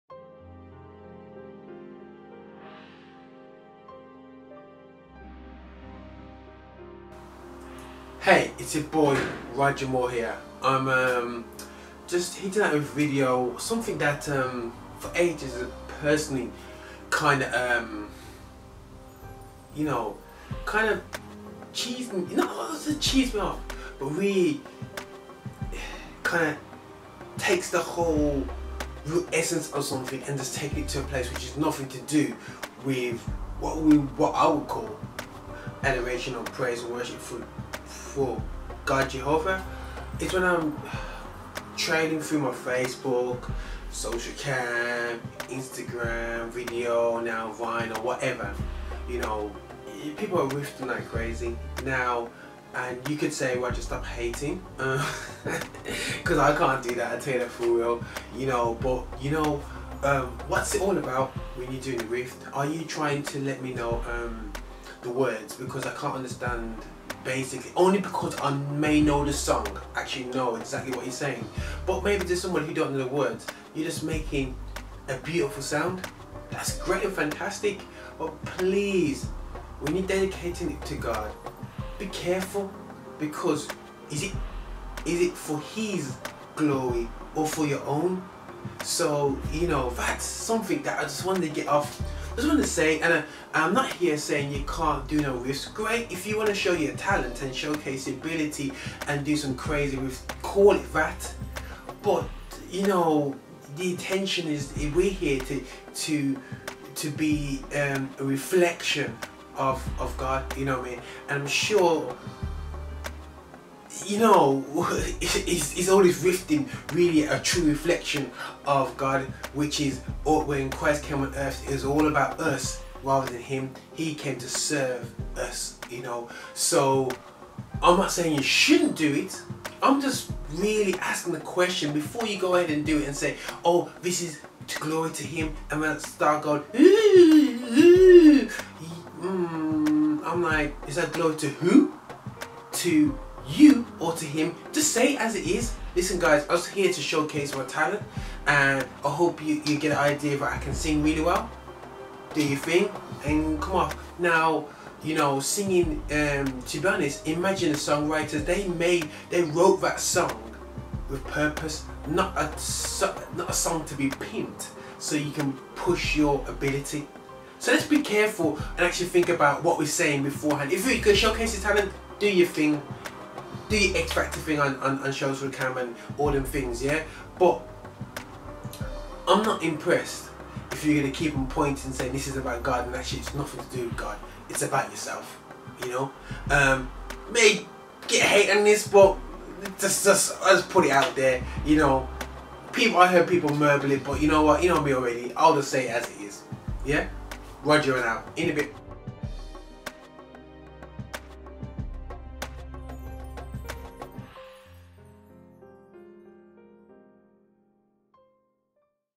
Roger Moore Vlog #30 - Annoying Vocal Riffs [so-called praise]
Roger Moore the visionary for GL360 shares his opinion on Annoying Vocal Riffs this has been dubbed as praise to God..